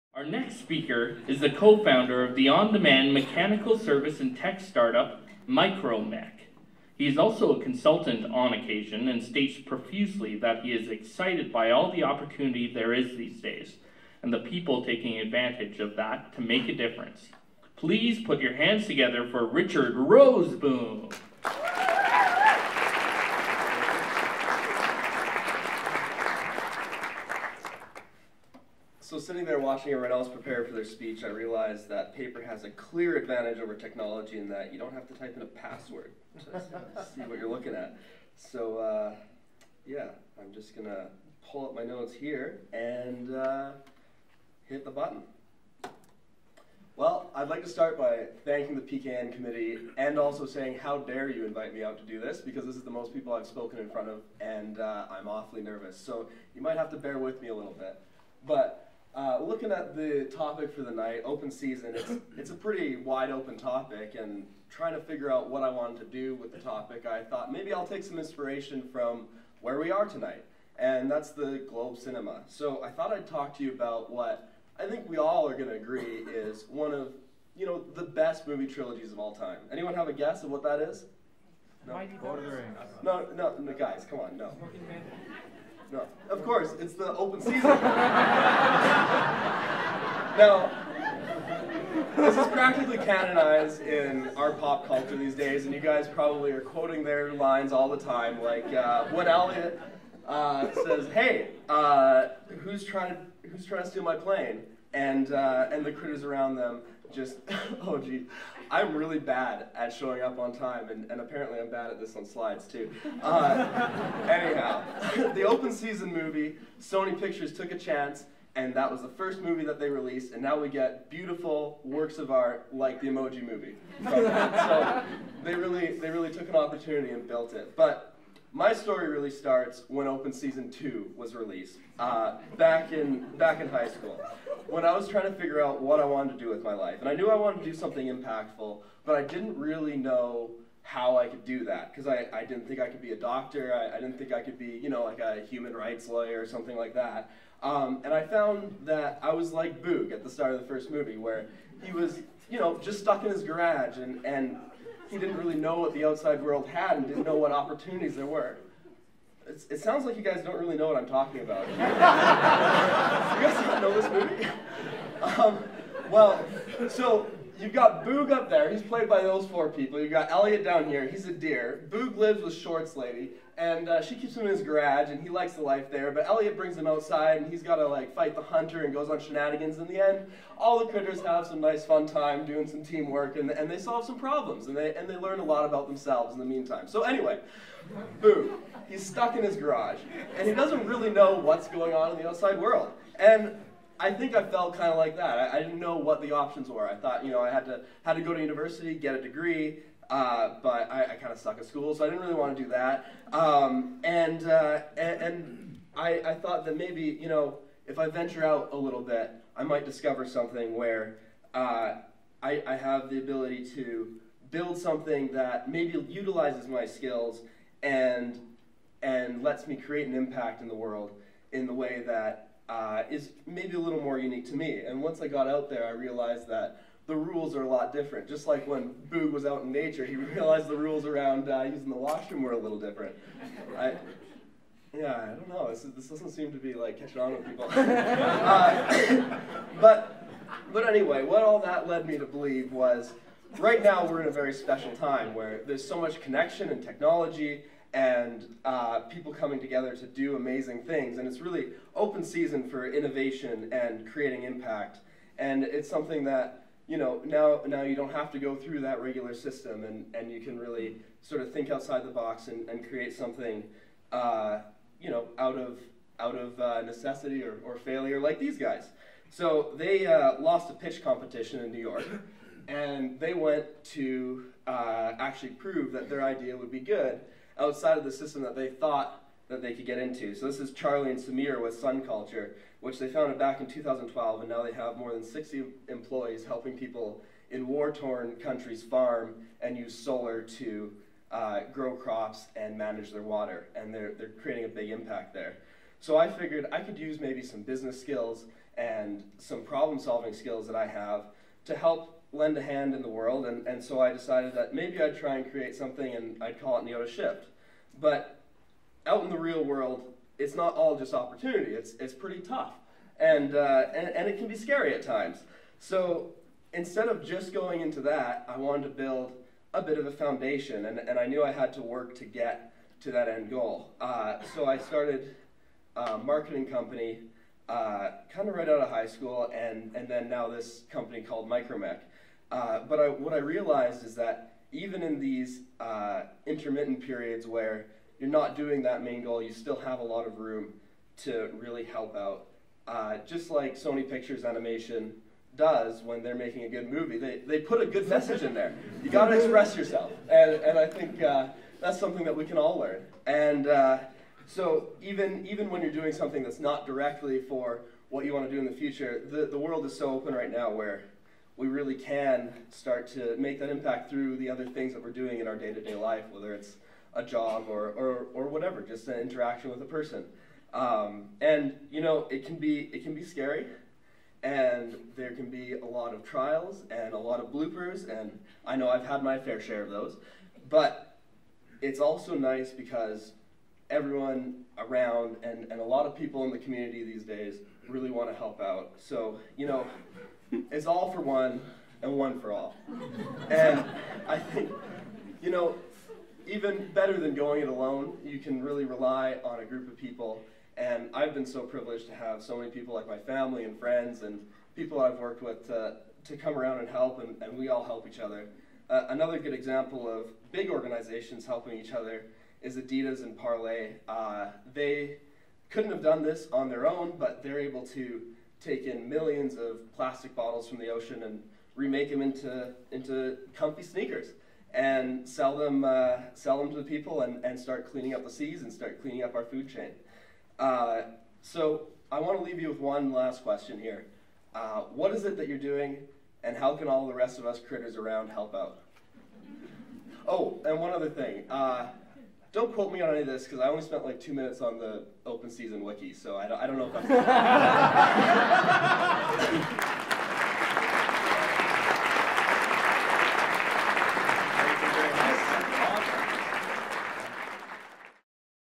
Richard Roseboom - PechaKucha Night Calgary #30: Open Season
Founder at MicroMech and consultant on occasion; Richard Roseboom speaks at PechaKucha Night Calgary #30: Open Season.